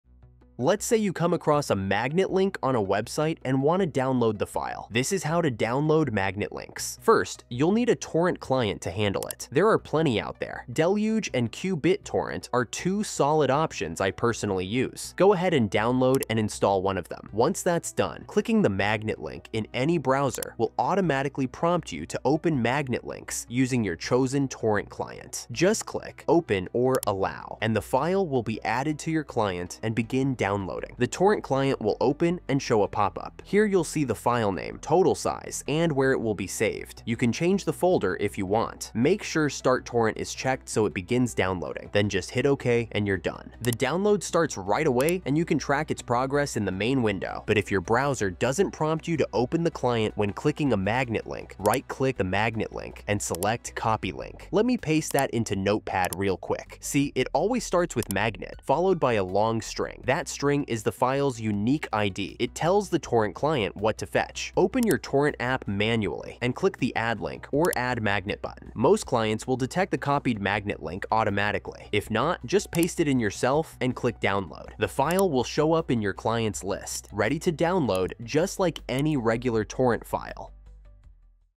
How to Download Magnet Links The Right Way
⚡This is how to download magnet links the easy way, fast and easy.
Whether it’s a file you found online or shared by a friend, magnet links are one of the simplest ways to download content via peer-to-peer sharing. In this quick tutorial, learn exactly how to download magnet links using free tools that work on any PC.

✅ What magnet links actually do and how they differ from torrent files
✅ Which free torrent clients to use (Deluge and qBittorrent recommended)
✅ What to expect when you click a magnet link—how your client handles it
✅ What to do if nothing happens: how to manually paste and download the link
✅ How to change download folders and track your progress easily

🎥 Sections:
0:00 How to Download Magnet Links
0:06 Pick a Torrent Client (qBittorrent or Deluge)
0:17 Opening Magnet Links in Browser
0:47 Manually Add Magnet Links If Browser Doesn’t Prompt